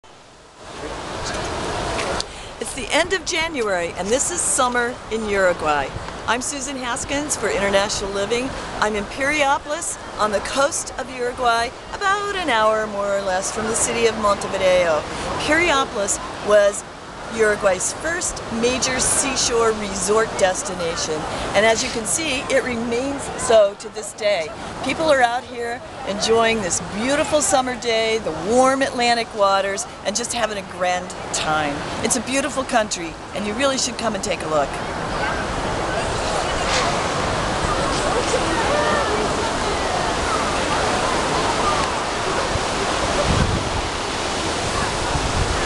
Uruguay: Summer Fun on the Beach in Piriapolis - International Living
Piriapolis, Uruguay: Summer Fun on the Beach - International Living

In the summertime, Piriápolis is full of vacationers who come to enjoy the friendly ambiance and stately old buildings, the nicely kept waterfront boardwalk and busy seafood restaurants, casinos, and of course, the beaches.